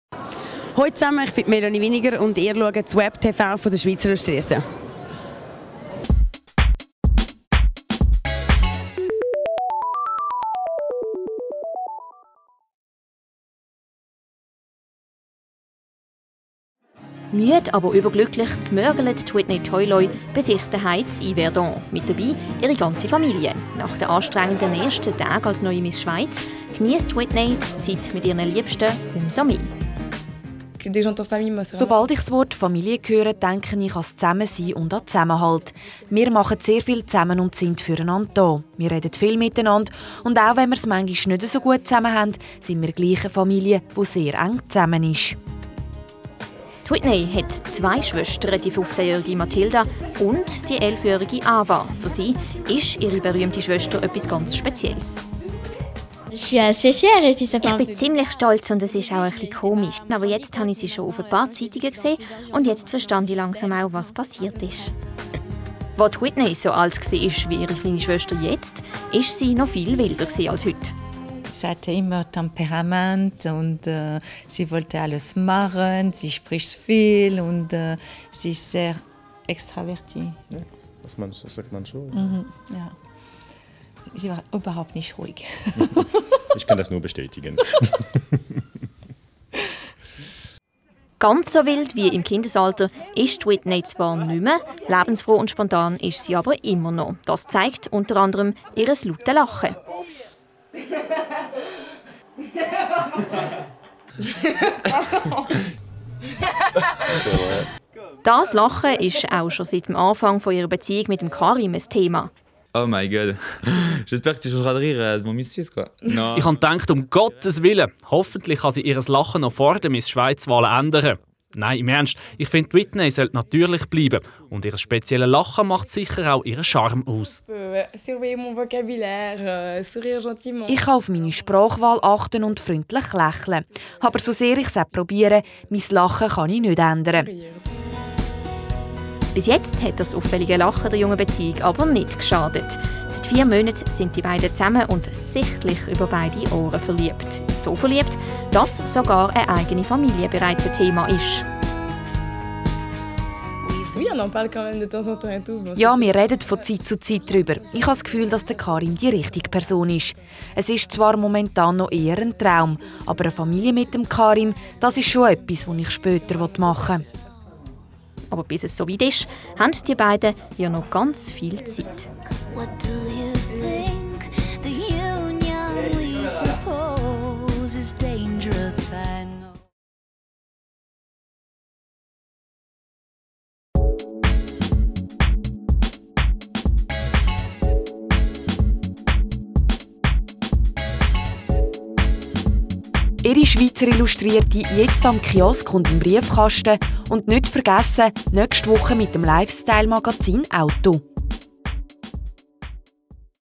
Die Romande ist der neue Liebling der Nation
- Whitney Toyloy - Zu Hause bei der neuen Miss Schweiz herrscht Aufregung. Whitney ist zu Hause. Zum ersten Mal seit ihrer Wahl zur Miss Schweiz. Ein Hausbesuch. (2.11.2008)